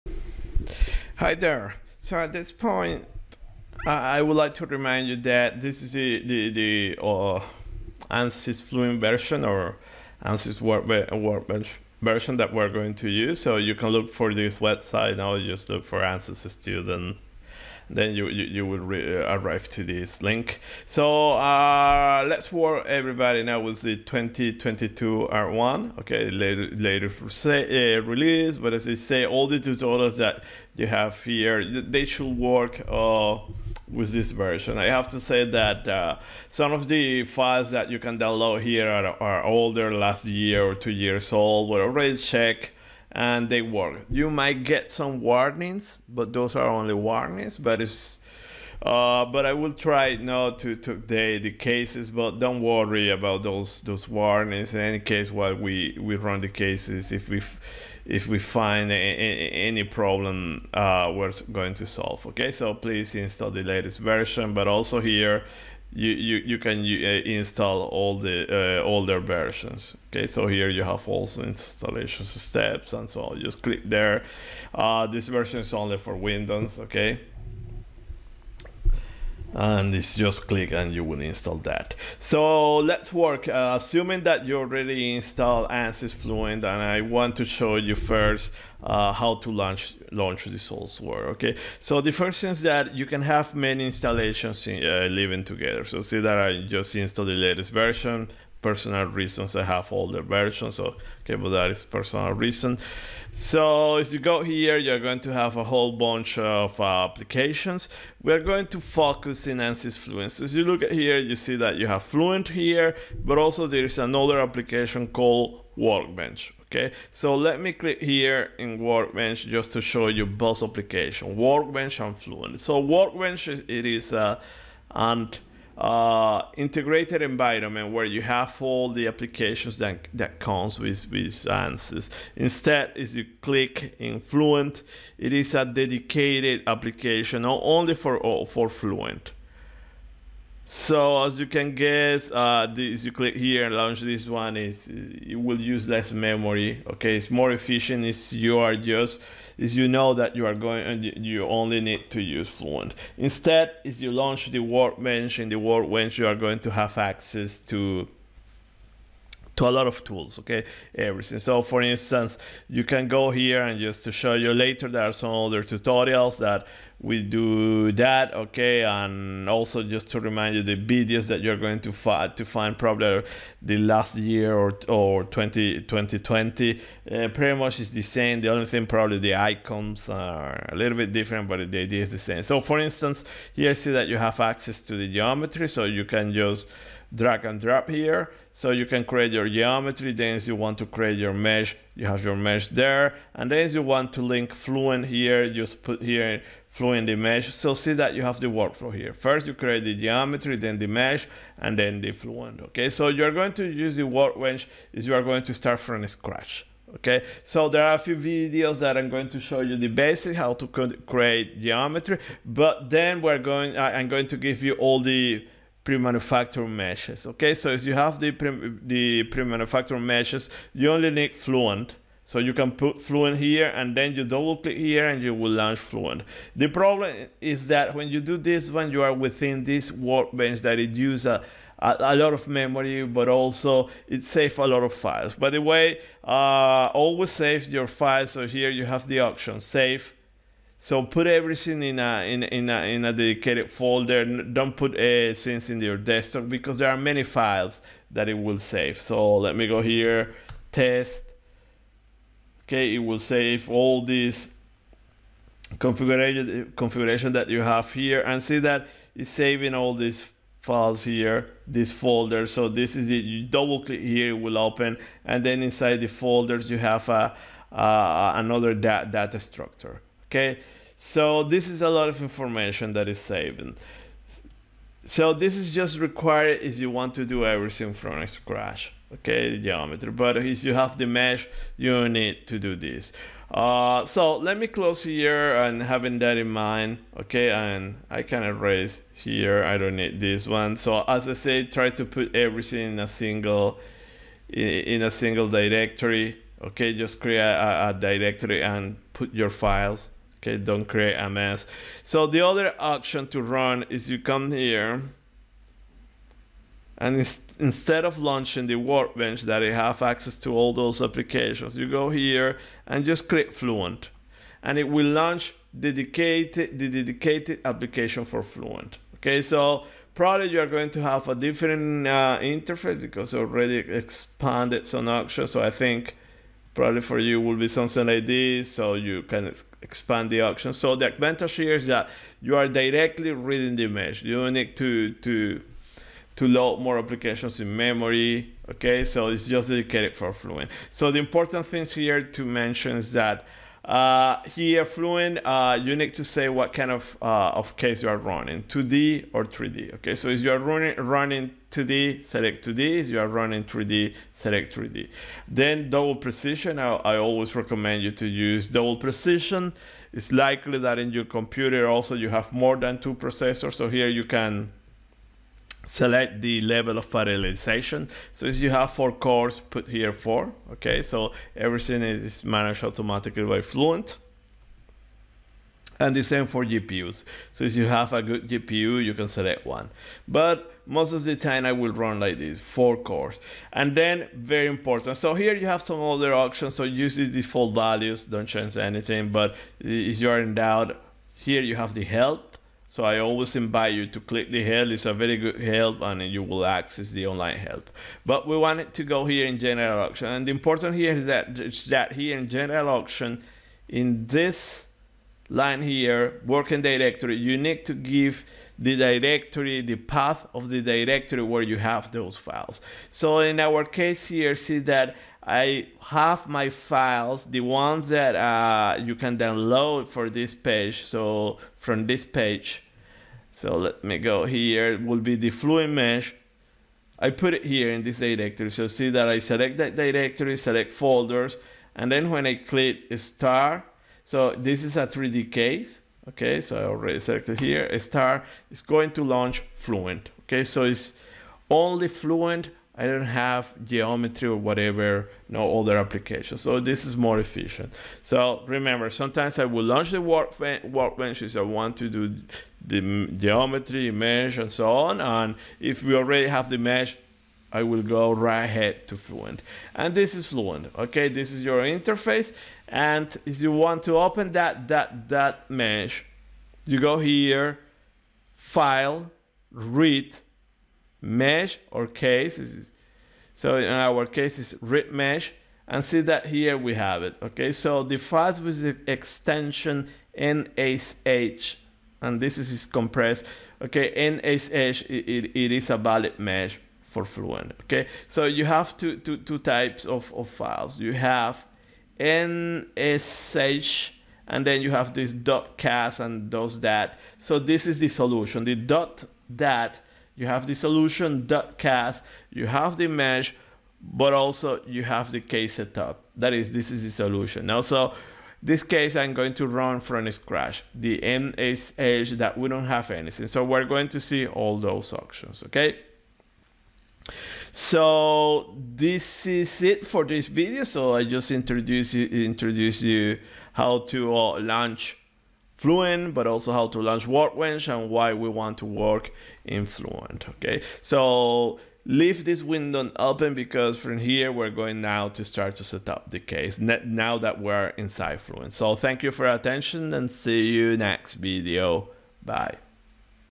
Tutorial 0 - Part 2 - My first tutorial
UNIGE 2022 - Turbulence and CFD Models course

Turbulence modeling using Fluent.
Tutorial 0.
Part 2. Launching Ansys Workbench and Ansys Fluent stand-alone